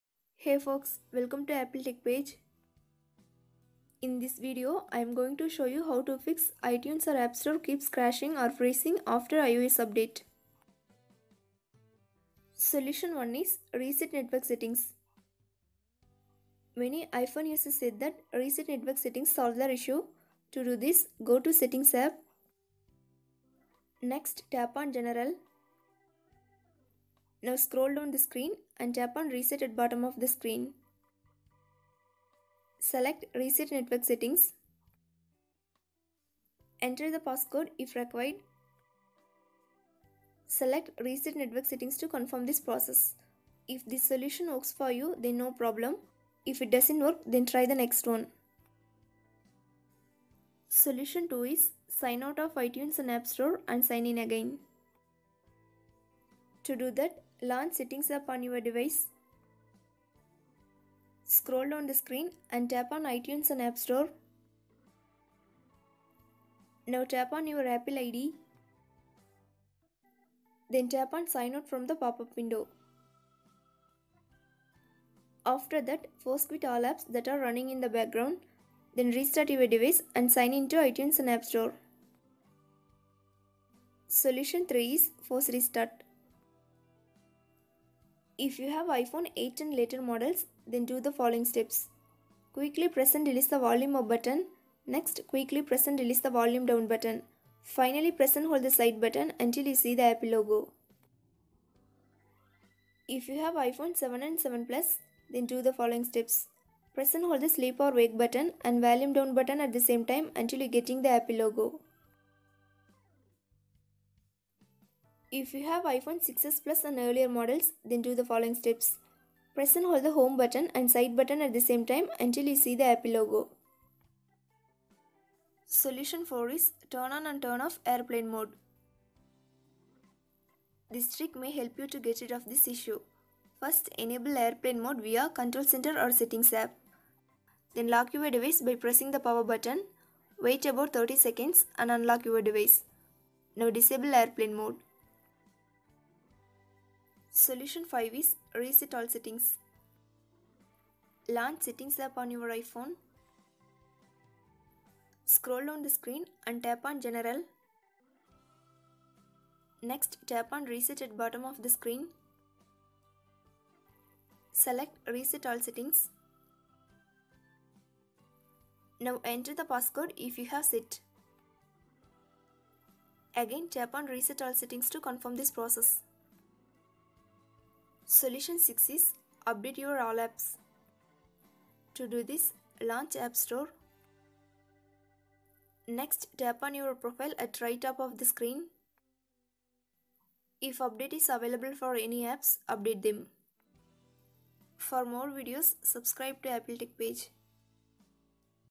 iTunes & App Store Keeps Crashing or Freezing on iPhone and iPad after iOS 13/13.4 - Fixed
how to fix iTunes and App Store keeps freezing in iOS 13, Cannot open iTunes and App Store, iTunes and App Store keeps crashing on iPhone x, XS and XS Max, iPhone 11 Pro max iTunes and App Store keeps freezing.